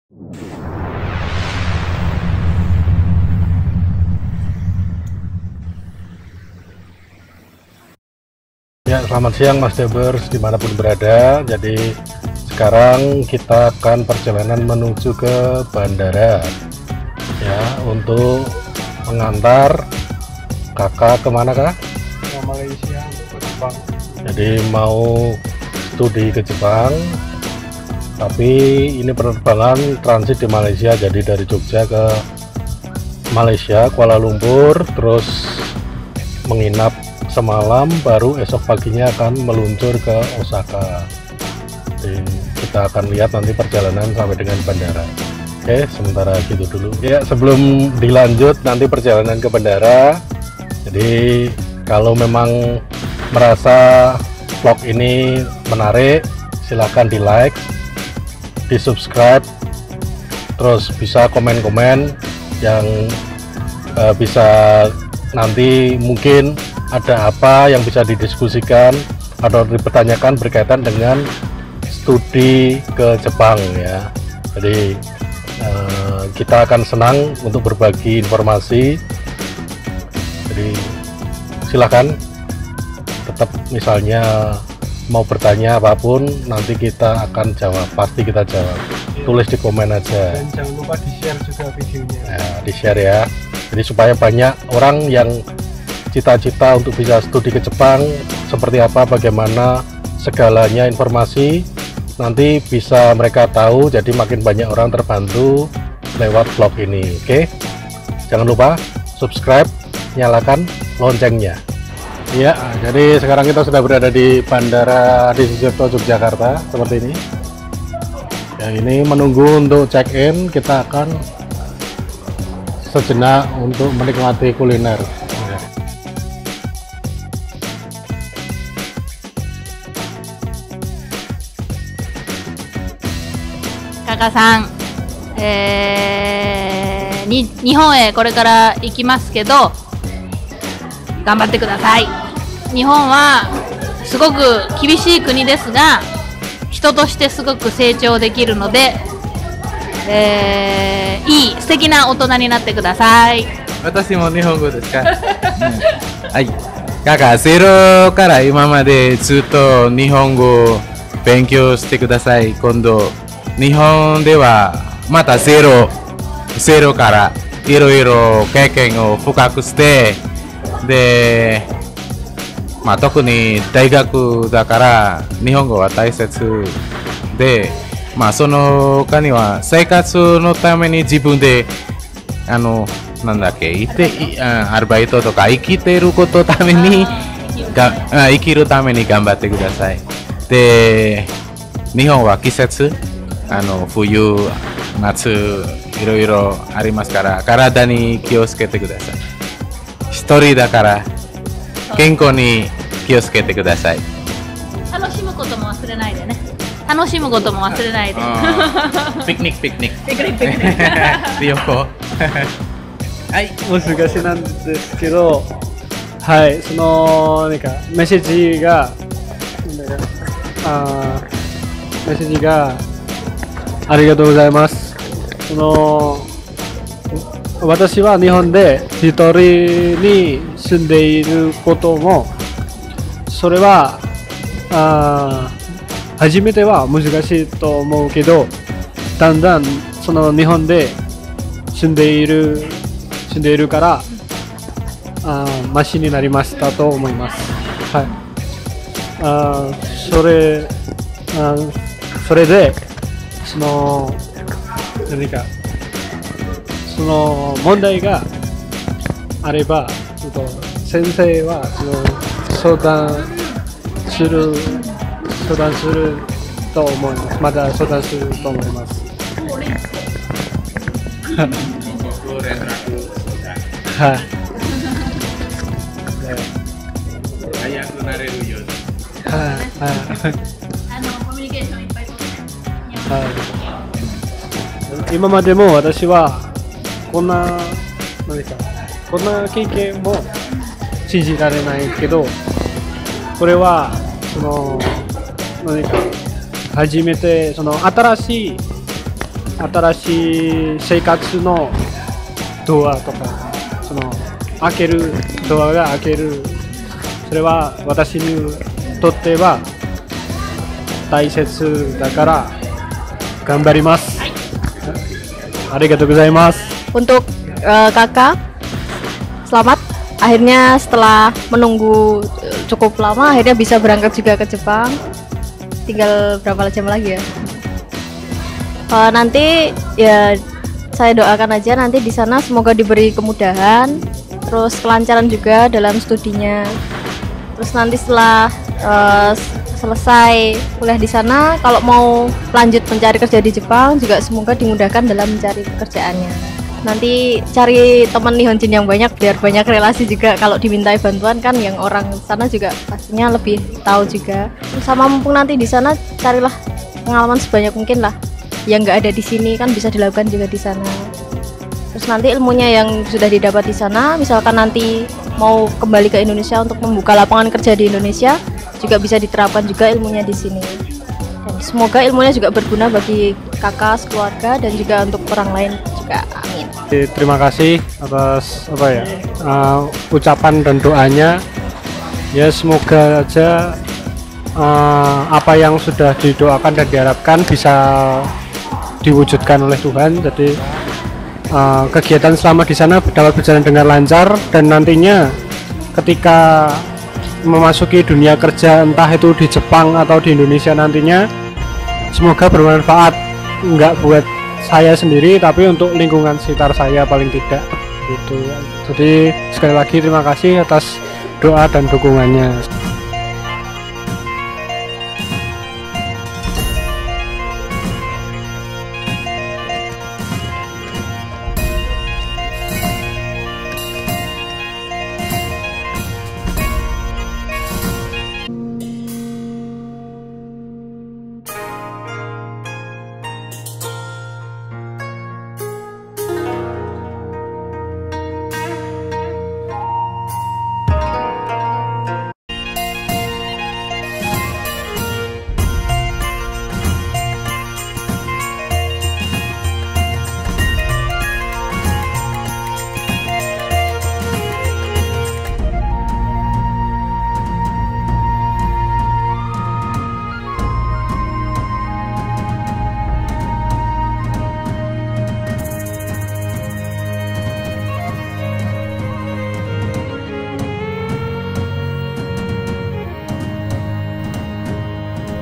AIR MATA DEMI MERAIH CITA-CITA | TRAVELER VLOG
Perjalanan panjang seorang anak entrepreneur muda handicraft untuk bisa kuliah di luar negeri (Kobe-Jepang)

Bila ada pertanyaan lebih lanjut tentang proses kuliah di luar negeri khususnya Jepang, bisa tulis di kolom momentar dan pasti akan dijawab.